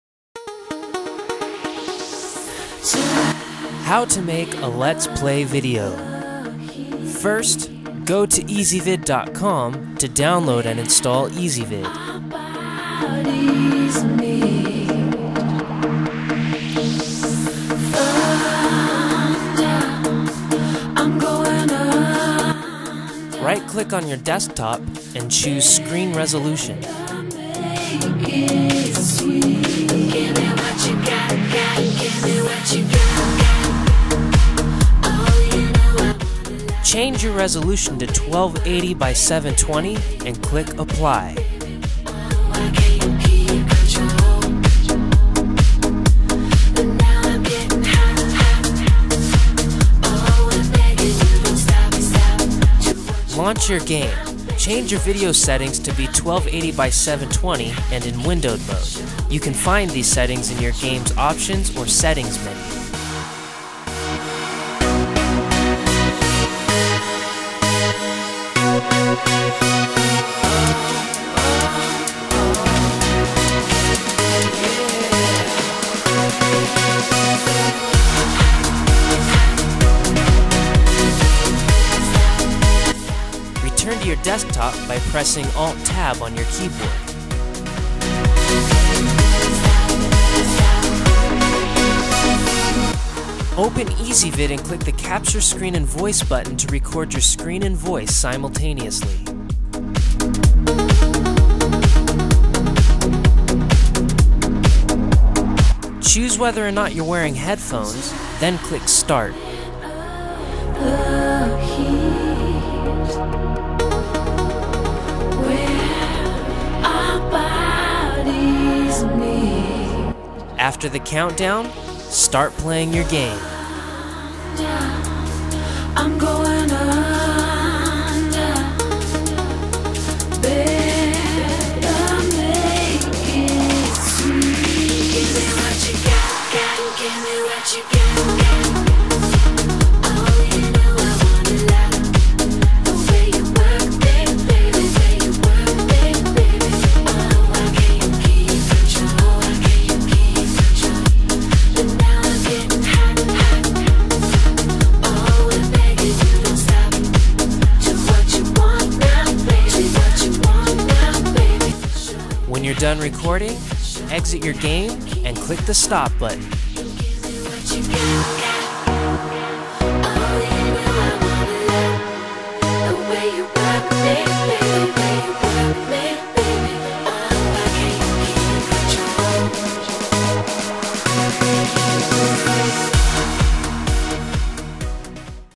How to make a "Let's Play" video
made with ezvid Make your own Let's Play videos of your favorite games using ezvid - a free screen recorder and video editor for Windows.  Record any game with your own live narration for free!